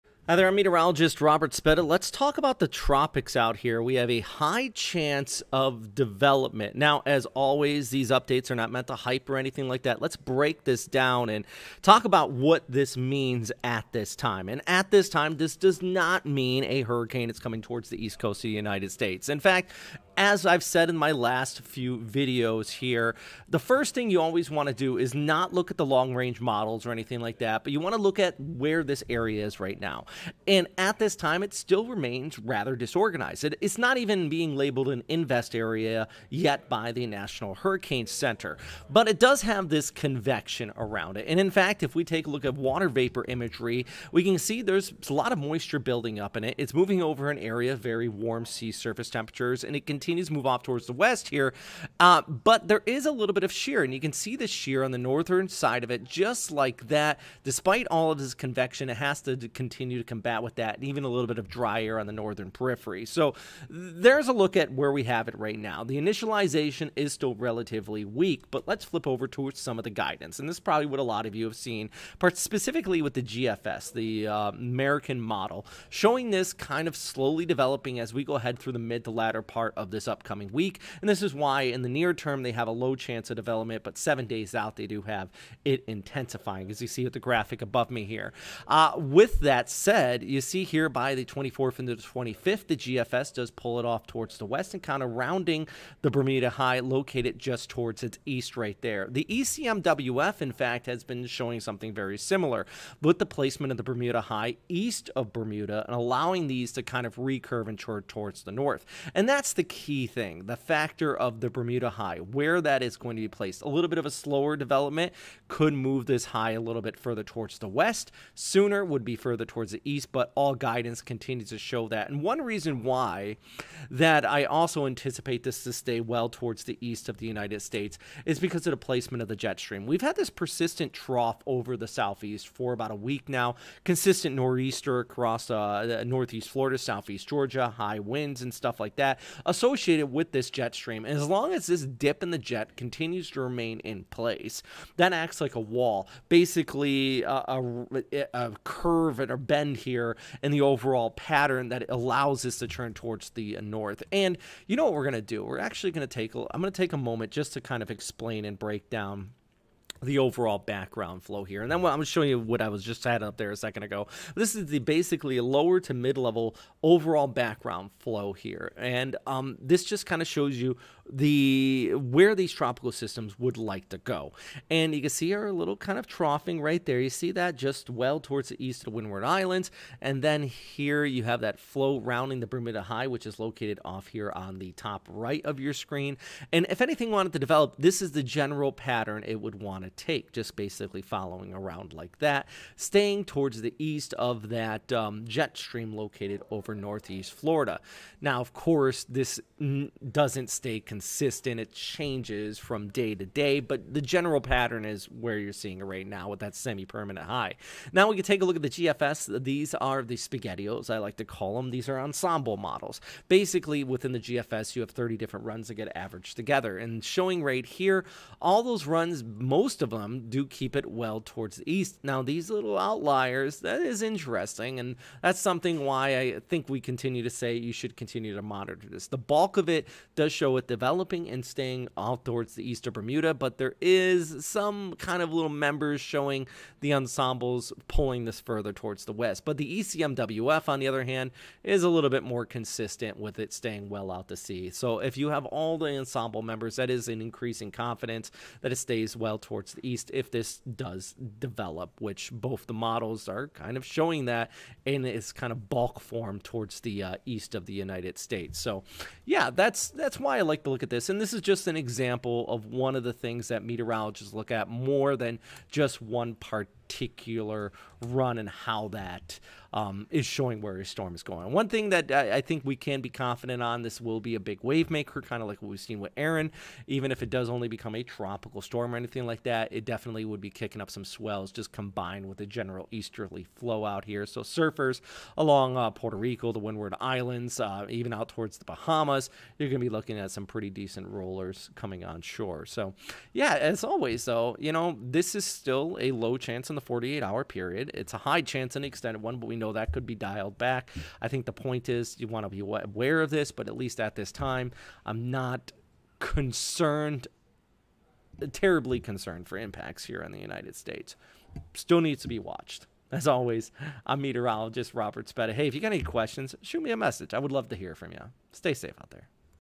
High chance of development in the Atlantic, what it means for the forecast
A look at the tropics and the area being monitored by the NHC for development